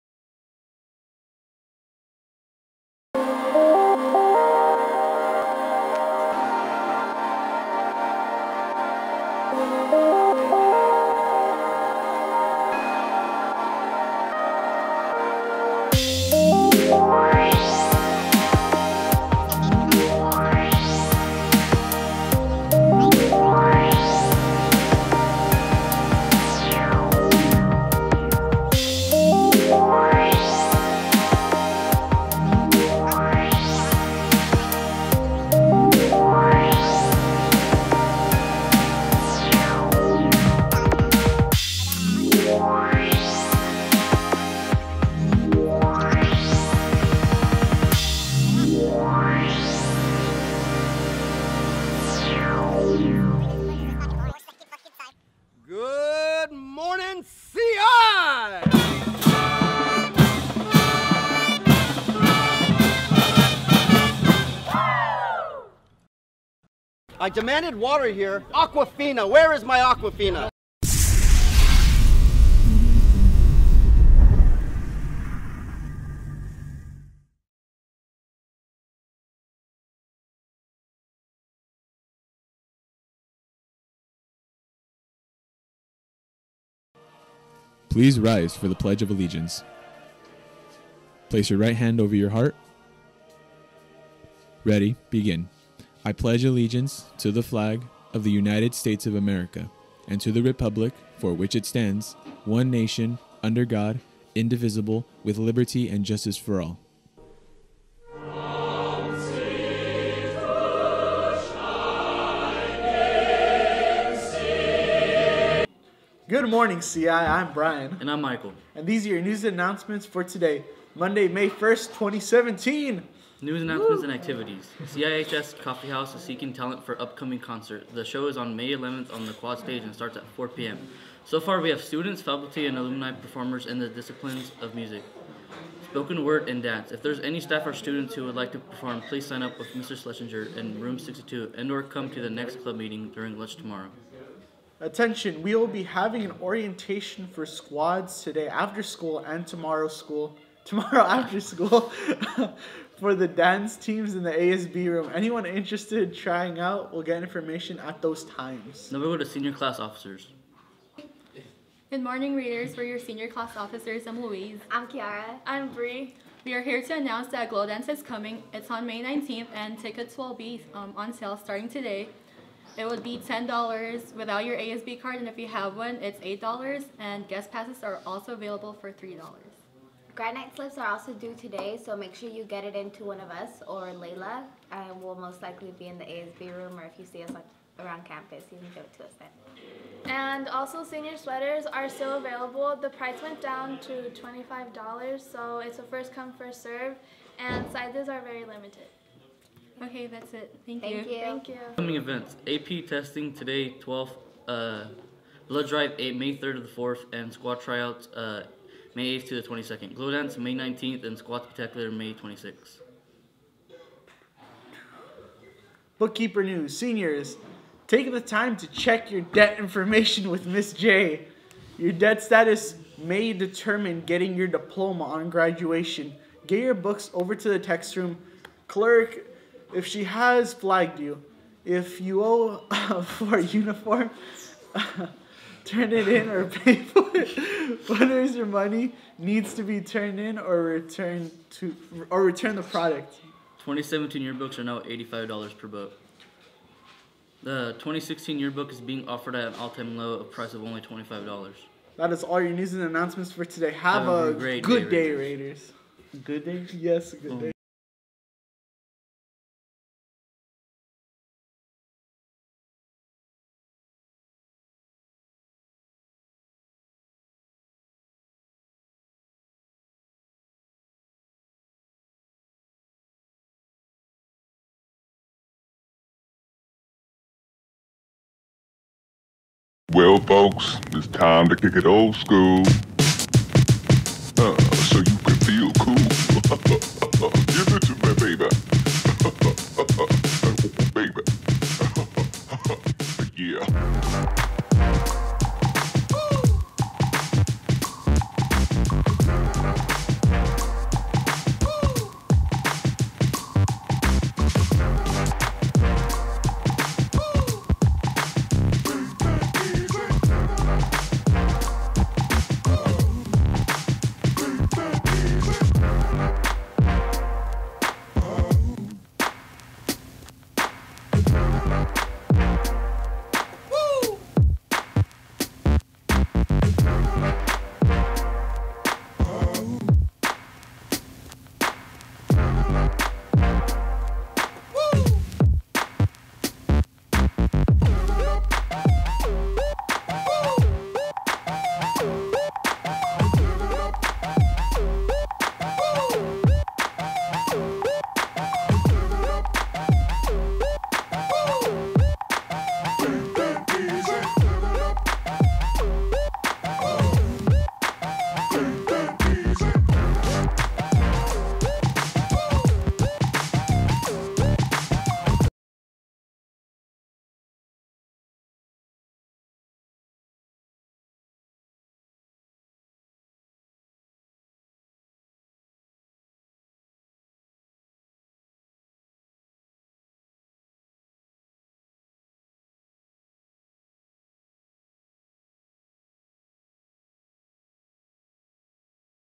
CI Pre-Recorded for Monday, May 1st, 2017
Only 6 weeks of school left, make sure you seniors are on track to graduate! Today a message from your Senior class officers, plus a new student made Filmstrip animation by A. Chavez. Happy May Day!